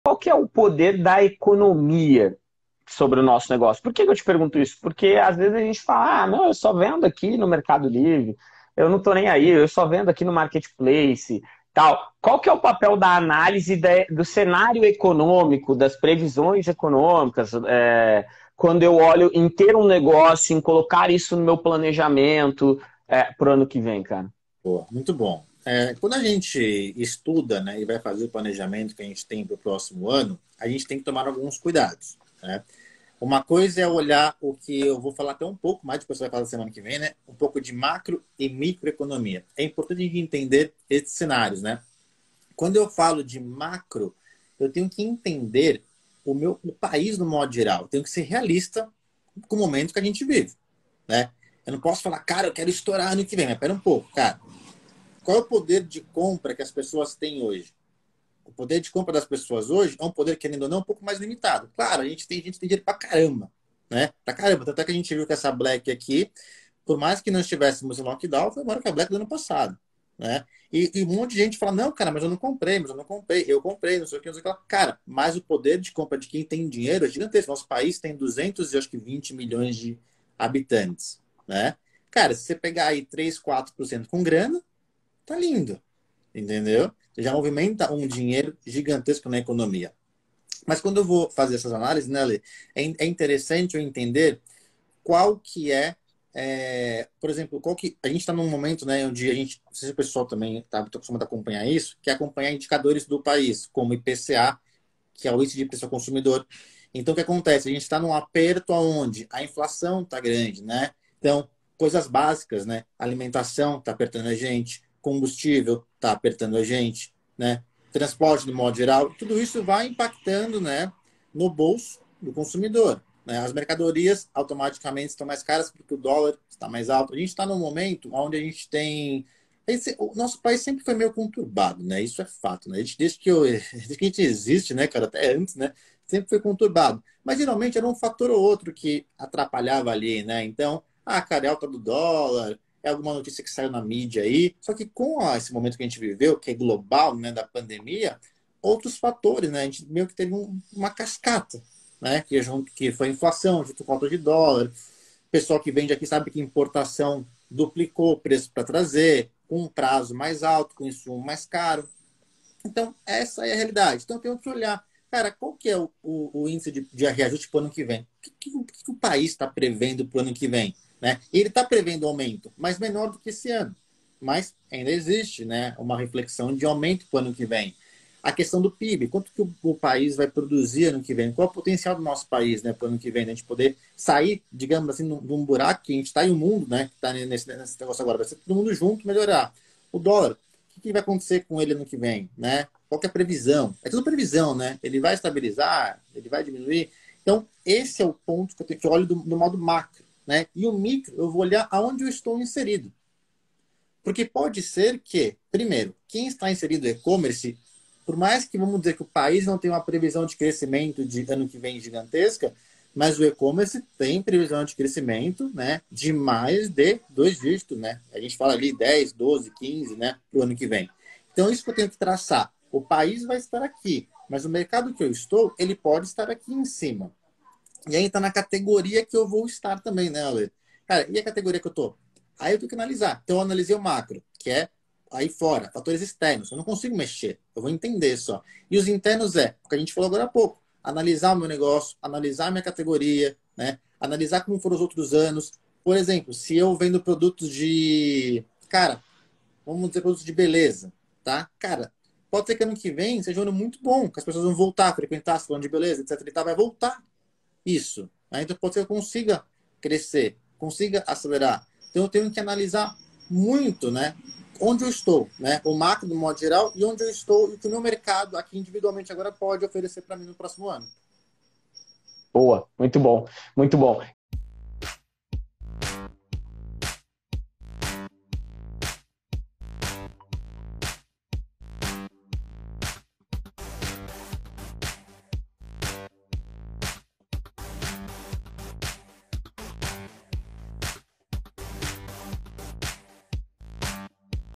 Como olhar o cenário econômico em relação ao seu negócio?
Trecho da live onde Alexandre Nogueira conversa com Felipe Arsilo sobre a importância de saber as tendências da economia para ajudar o seu negócio.

🤝Fale com a gente para nós ajudarmos as suas vendas crescerem

CUPOM: UNIMKT10
Ecomonize com o Frete no Melhor Envio:

CUPOM: PARCEIRO20